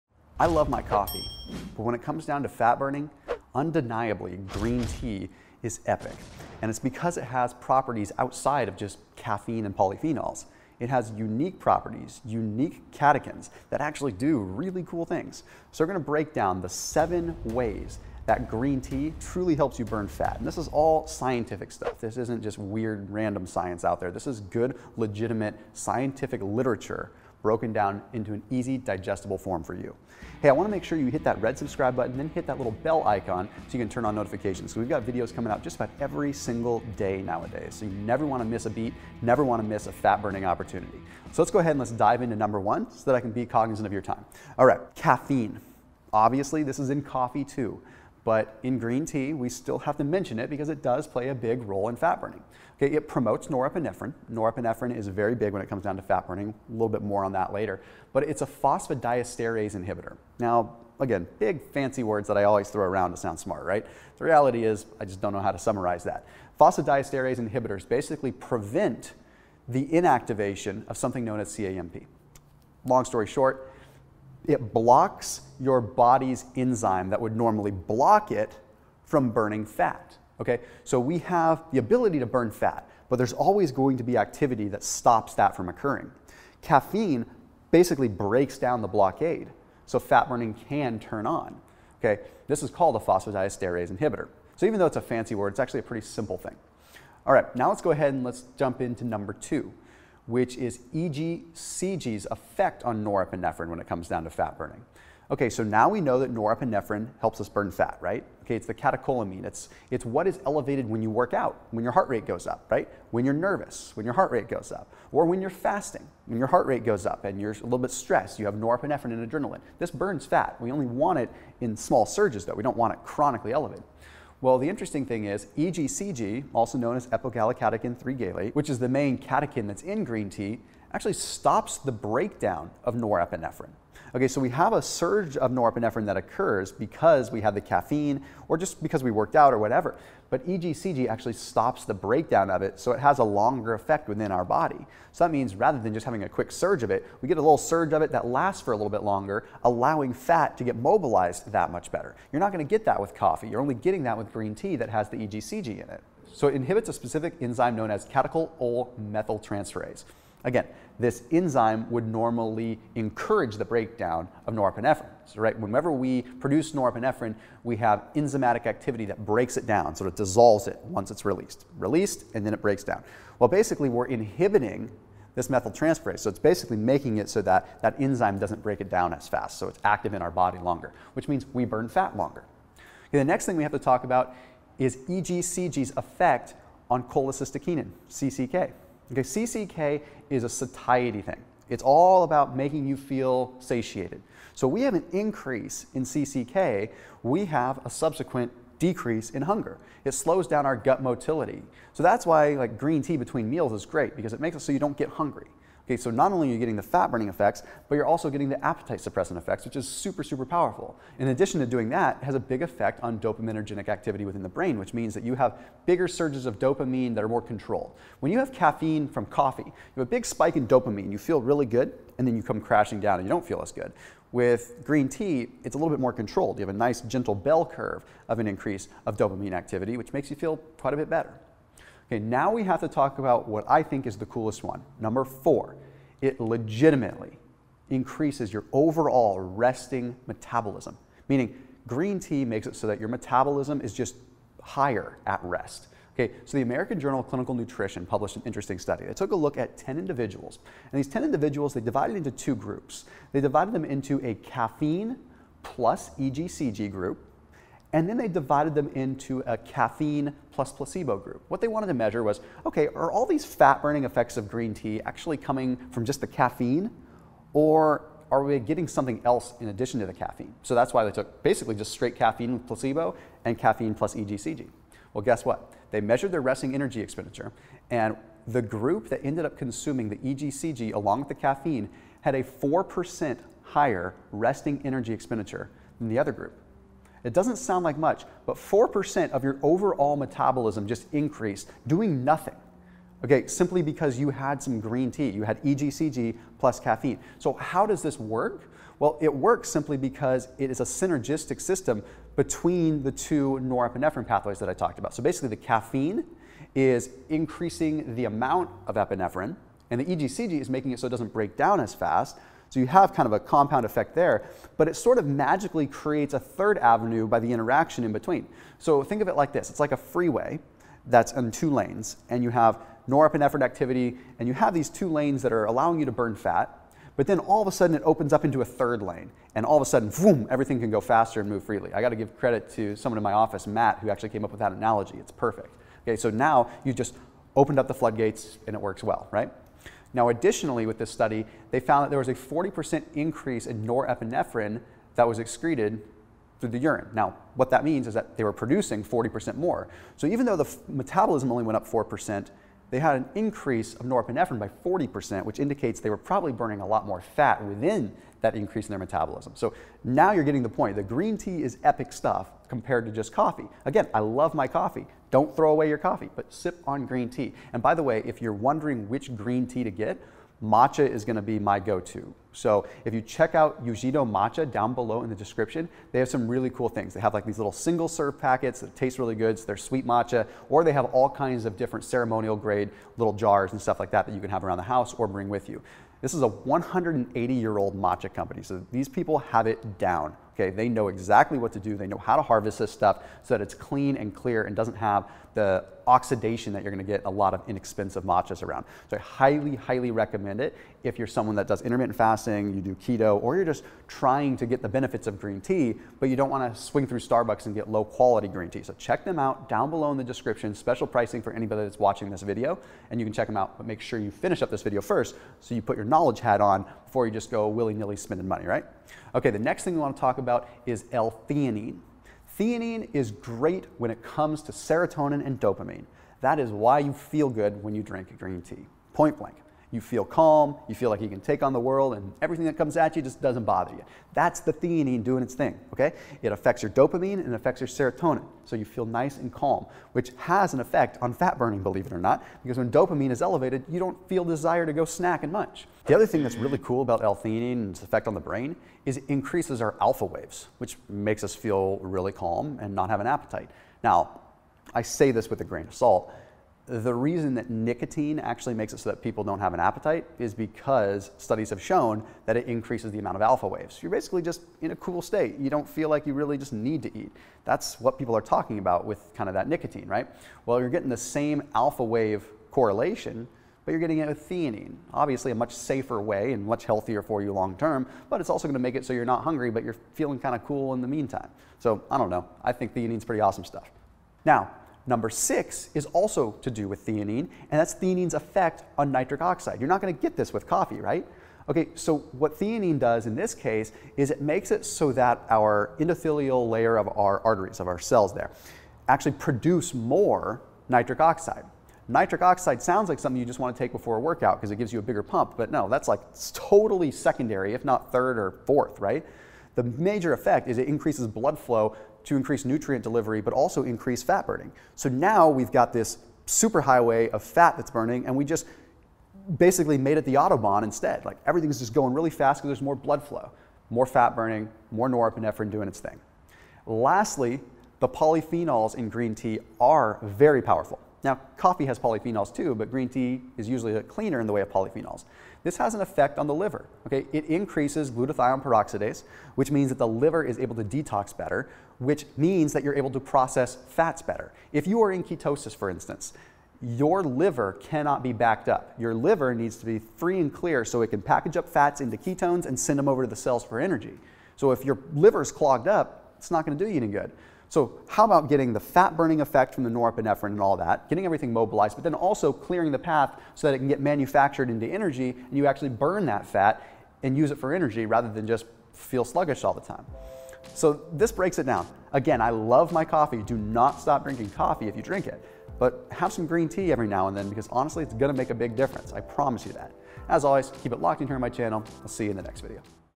7 Ways Green Tea Burns Fat- More Than Coffee…???
7 Ways Green Tea Burns Fat- More Than Coffee…??? - Thomas DeLauer

Caffeine

Caffeine is a phosphodiesterase inhibitor - phosphodiesterase inhibitor prevents the inactivation of the intracellular second messengers cyclic adenosine monophosphate (cAMP)

cAMP activates hormone sensitive lipase, promoting lipolysis and increasing FFA’s

EGCG & Norepinephrine

Green tea contains a phytonutrient called EGCG can help inhibit an enzyme that breaks down the hormone norepinephrine

When this enzyme is inhibited, the amount of norepinephrine increases, promoting fat breakdown

Specifically, it inhibits catechol-O-methyltransferase, which is an enzyme that degrades catecholamines (such as dopamine, epinephrine, and norepinephrine) - inhibition of this enzyme prevents the degradation of catecholamines

CCK - Satiation

EGCG increases the hormone CCK (cholecystokinin), which is responsible for creating the feeling of satiation

CCK slows down intestinal motility as it causes food to move more slowly through the intestines - feel full for a longer period of time

Increased Metabolic Rate

Study - American Journal of Clinical Nutrition

Investigated whether green tea extract, by virtue of its high content of caffeine and catechin polyphenols, could increase 24-h energy expenditure (EE) and fat oxidation in humans.

Twenty-four-hour EE, the respiratory quotient (RQ), and the urinary excretion of nitrogen and catecholamines were measured in a respiratory chamber in 10 healthy men
On 3 separate occasions, subjects were randomly assigned among 3 treatments: green tea extract (50 mg caffeine and 90 mg epigallocatechin gallate), caffeine (50 mg), and placebo, which they ingested at breakfast, lunch, and dinner.

Relative to placebo, treatment with the green tea extract resulted in a significant increase in 24-h EE (4%)

Twenty-four-hour urinary norepinephrine excretion was higher during treatment with the green tea extract than with the placebo (40%)

Treatment with caffeine in amounts equivalent to those found in the green tea extract had no effect on EE and RQ nor on urinary nitrogen or catecholamines

L - Theanine: Serotonin & Satiation

L-theanine increases brain levels of serotonin, dopamine, GABA levels - serotonin acts to increase cholecystokinin and leptin, giving you a “full” feeling

This is a weaker link, but theanine increases levels of alpha waves, putting you in a focused state of mind - you’d likely be less inclined to eat during this, although there isn’t a published study looking directly at this

Although there are some studies show that the appetite reducing effects of nicotine are because it increases alpha waves

L-Theanine: Nitric Oxide

Theanine increases blood flow as it increases the production of nitric oxide in endothelium cells (cells that line the interior surface of blood vessels and lymphatic vessels), which is a gas that widens blood vessels and improves blood flow

It does this by activating the phosphorylation of eNOS which regulates NO generation

A wider, more dilated blood vessel allows for greater blood to flow through it, which means more nutrient rich blood is transported to your muscles, delivering the essentials it needs to repair and grow.

Theanine increases blood flow, which may increase the delivery of fat burning hormones like epinephrine and norepinephrine

Liver Health

Polyphenols in green tea can help improve liver health - it lowers lipid levels in the body and reduces the risk of non-alcoholic fatty liver disease

Polyphenols enhance the liver’s ability to produce glutathione - Phase II liver detoxification neutralizes the byproducts of phase I liver detoxification and other remaining toxins

Under normal conditions, phase 2 liver detoxification enzymes produce low levels of glutathione - under times of high toxic stress the body needs a higher production of glutathione